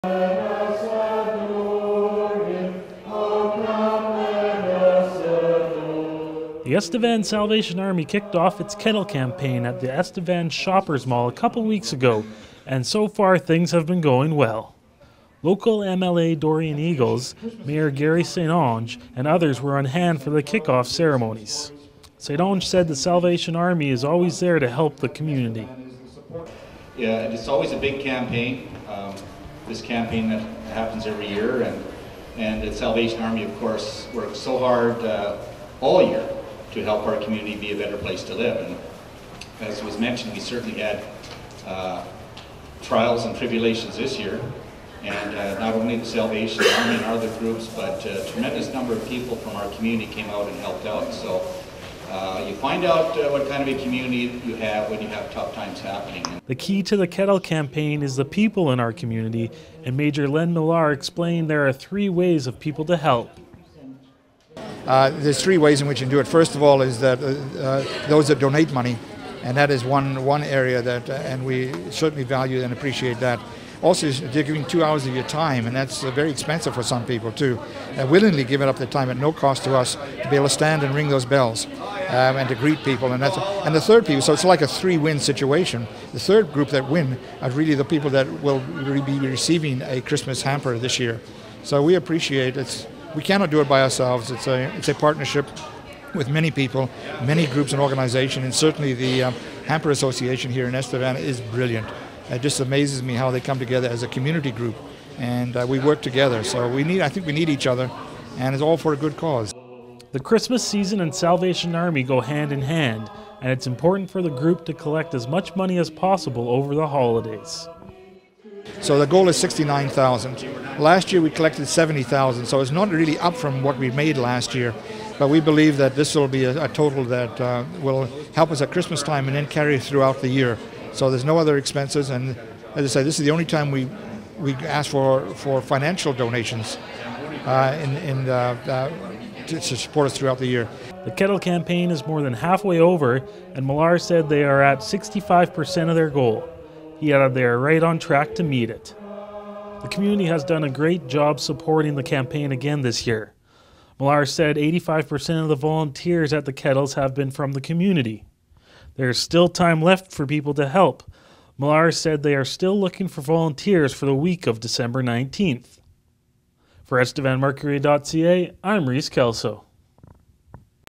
Kettle Campaign 2011
The Estevan Salvation Army kicked of its annual Kettle Campaign at the Estevan Shoppers Mall.